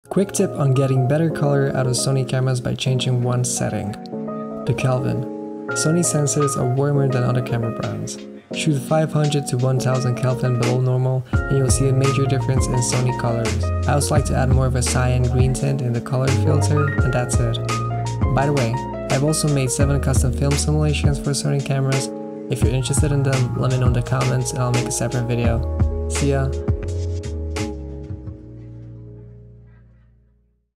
Fix Sony Colors only with white balance
Get perfect skintone out of sony cameras with this simple trick, the white balance
Sony has a warmer colors than most camera brands and you need to bring down thw white balance to get the correct white point.

Get Great Colors Out of Camera with the 7 film simulations for Sony Cameras sony a7iii and a7sIII ;)

Sony sensors have warmers tones, that's why we need to shoot 500-1000 Kelvin lower than normal
It's always important to get correct white balance right in camera, so you spend less time correcting in color grading

special thanks to my friend @eatbuildriderepair for being my model, go check his channel